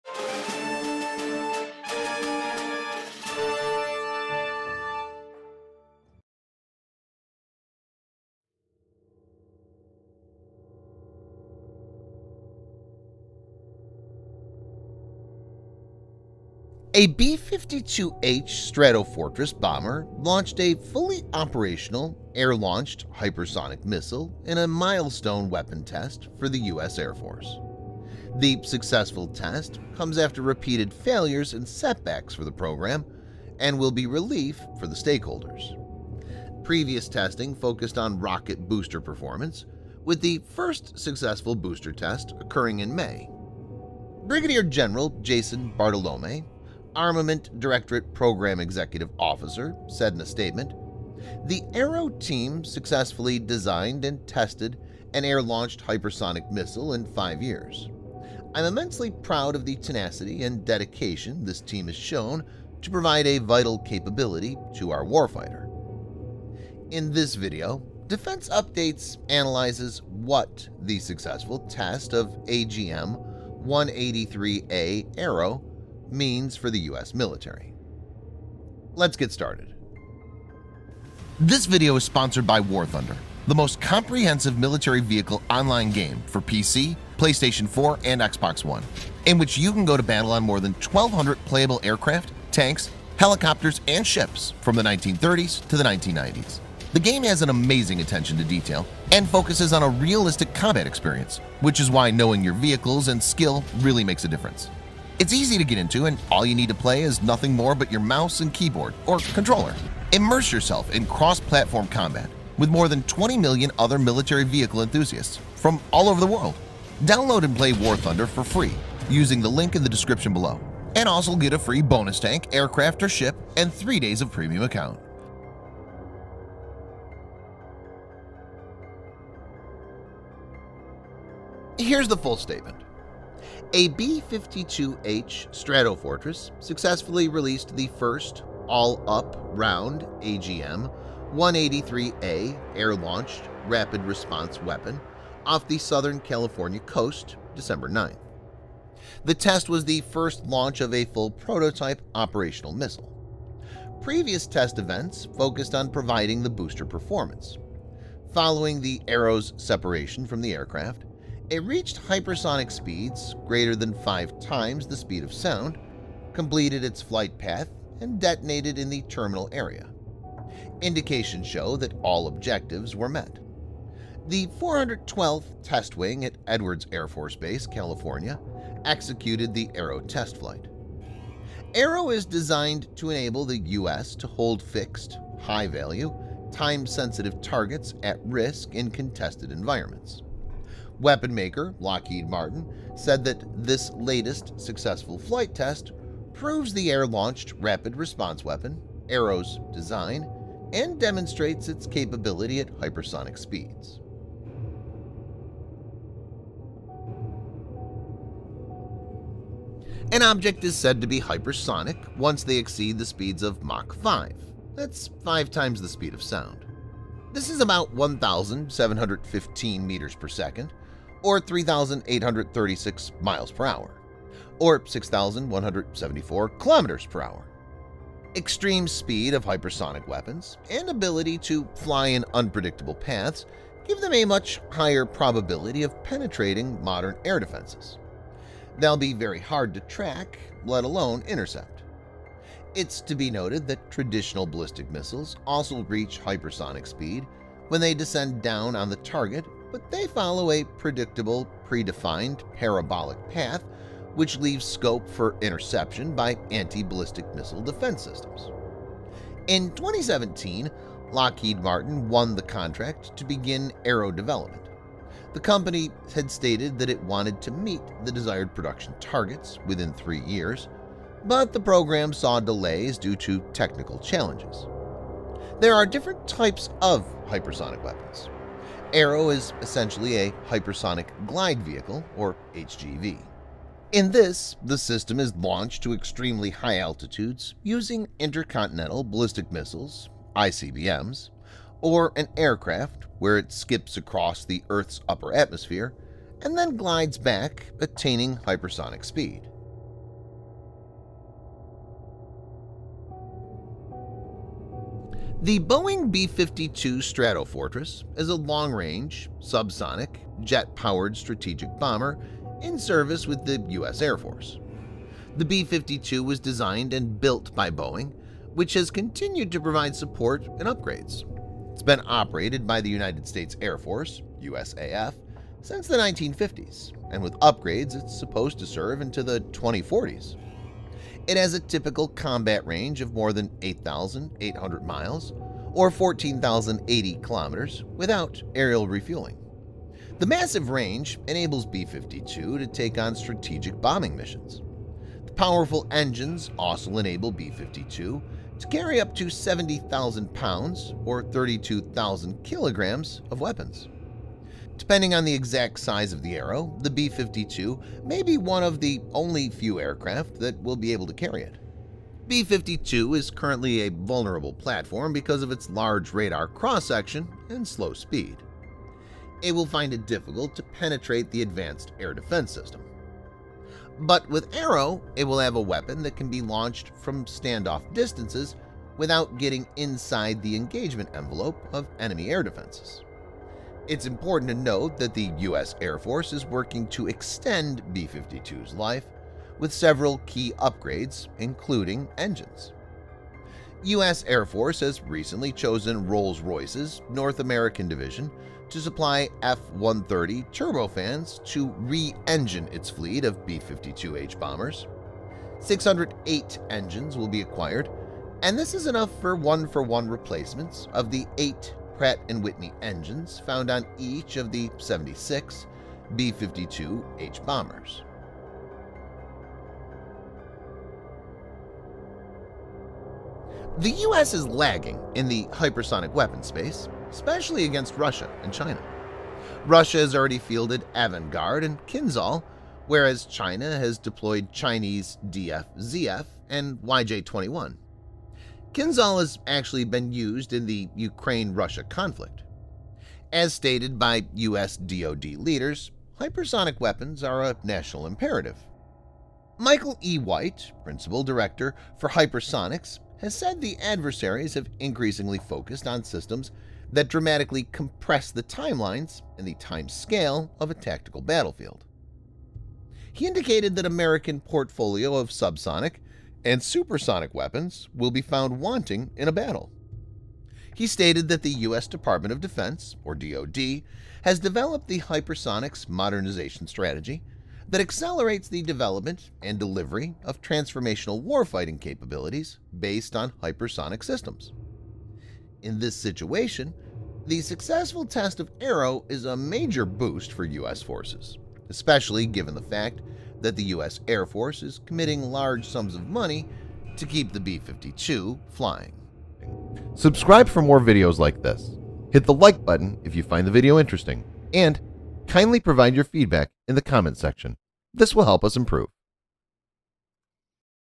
of USairforce- finally test is successful !
A B-52H Stratofortress bomber launched a fully-operational air-launched hypersonic missile in a milestone weapon test for the US Air Force.

The successful test comes after repeated failures and setbacks for the program and will be relief for the stakeholders.

Previous testing focused on rocket booster performance, with the first successful booster test occurring in May.

Brig. Gen. Jason Bartolomei, Armament Directorate Program executive officer, in the statement,
"The ARRW team successfully designed and tested an air-launched hypersonic missile in five years. I am immensely proud of the tenacity and dedication this team has shown to provide a vital capability to our warfighter."

In this video, Defense Updates analyzes what the successful test of AGM-183A ARRW mean for the US military ?

BACKGROUND MUSIC

IMAGES / VIDEO USED